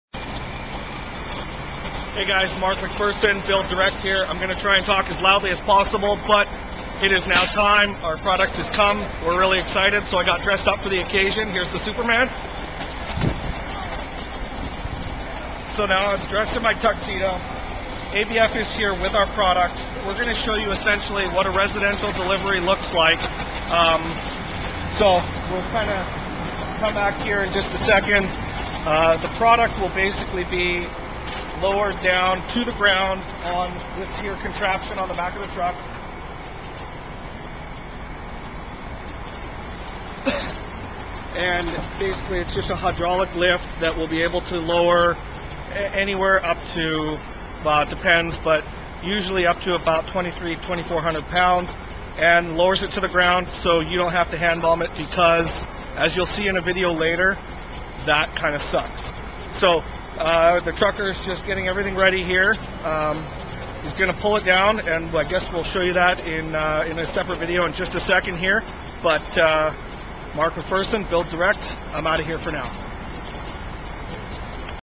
Laminate Flooring Delivery
The laminate flooring that we're going to wreck has arrived.  And while we're at it, BuildDirect describes what you can expect from a laminate flooring delivery.